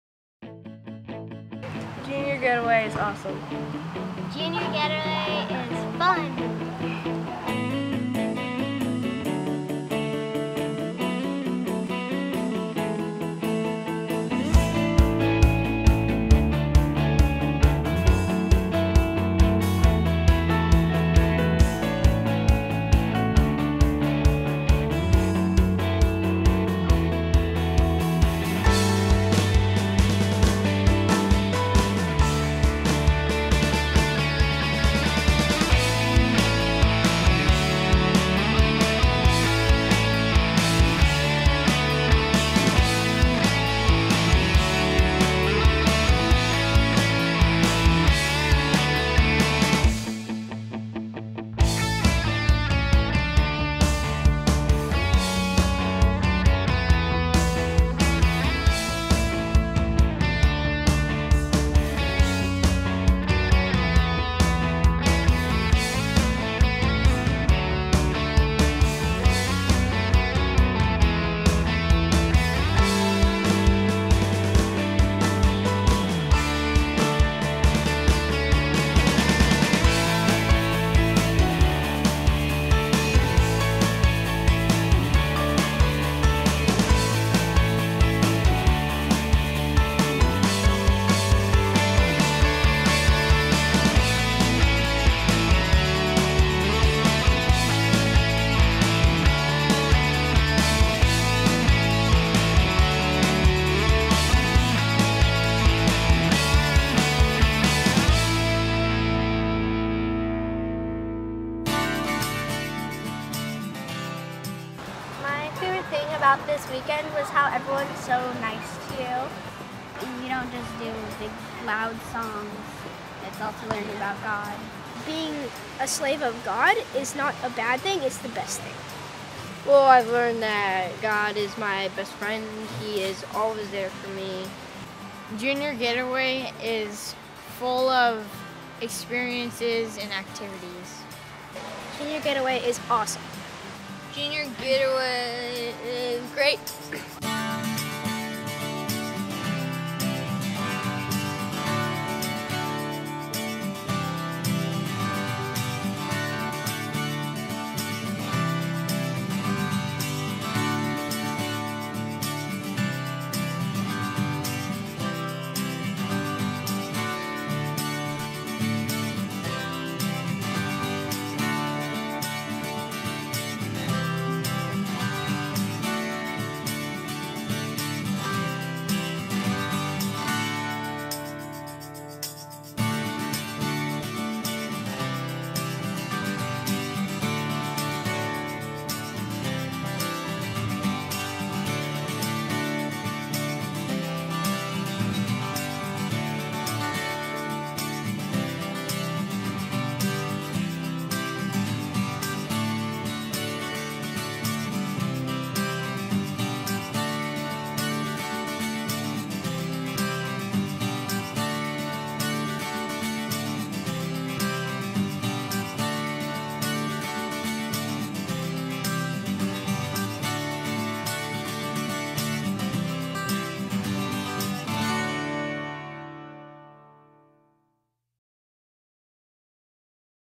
2012 Junior Getaway: March 16-18
Highlights from our second week of Junior Getaway at Mount Hermon. These weekends are a chance to experience the beauty of God's creation, make new friends, and be immersed in the life-transforming Truth. Join other 4th-6th graders as they experience camp in an intentional, life-changing way and embark upon the journey of a lifetime.